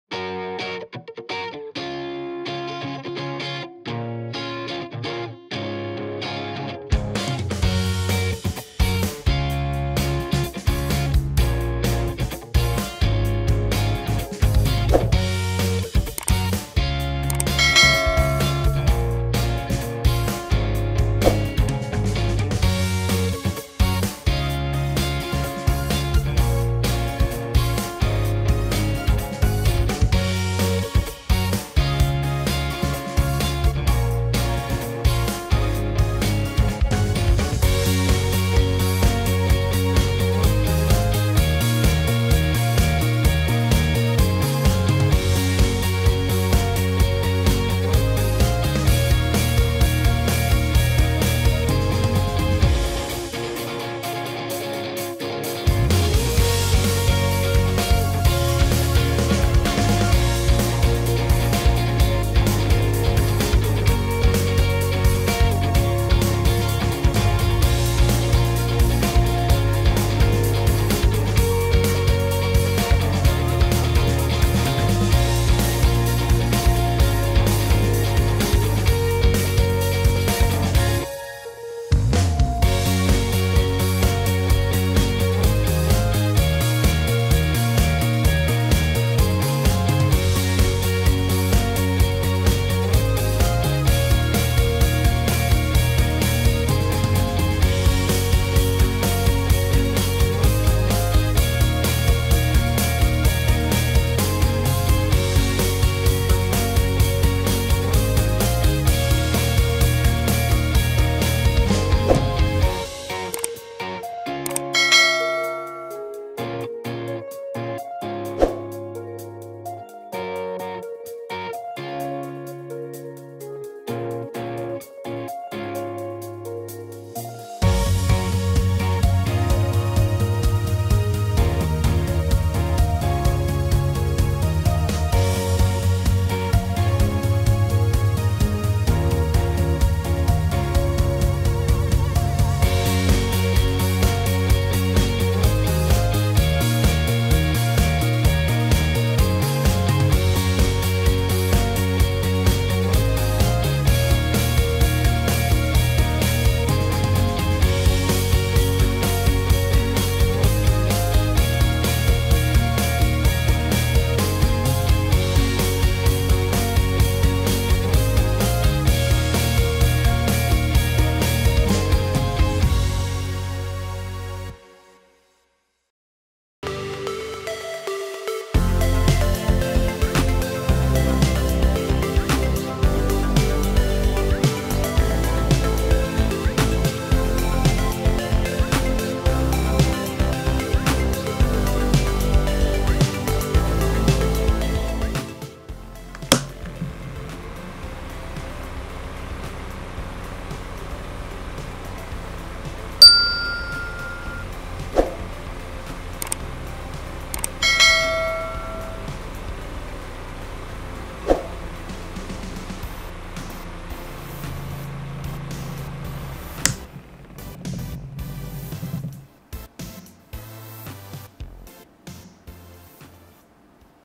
Summer Craft: Table Fan Out of Cardboard at Home - DIY Crafts
Hello fanatics! Today I show you how to make a summer table fan out of cardboard at home. Enjoy!